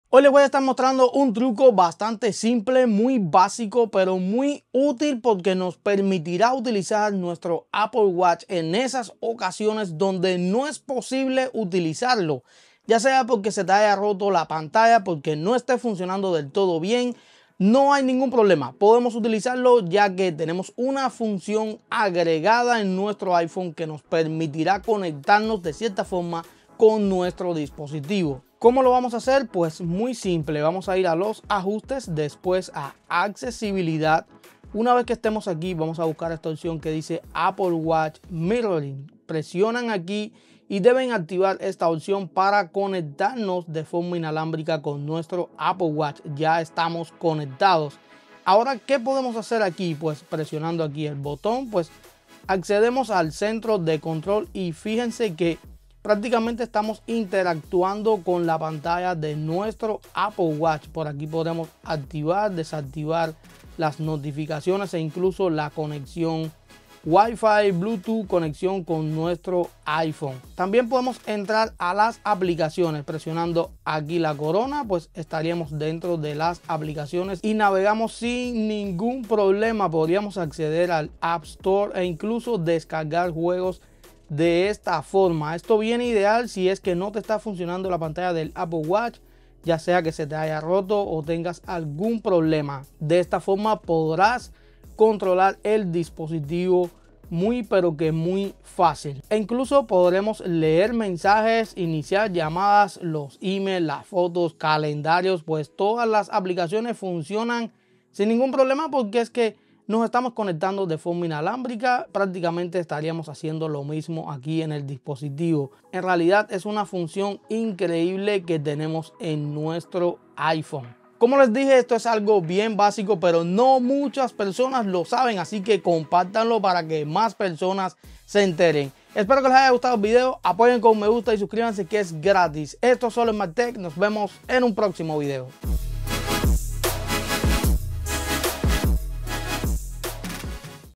CONTROLA Tu Apple Watch Desde el iPhone
En este Video Quiero Mostrarles una función que no todos saben que existe en nuestro iPhone para controlar nuestro Apple Watch

Capitulos
00:00 Introducción
00:20 Tenemos una Función
00:28 Como lo Vamos a Hacer
00:40 Deben Activar
01:00 Apple Watch
01:22 Descargar Juegos
02:00 Nuestro iPhone
02:21 Fin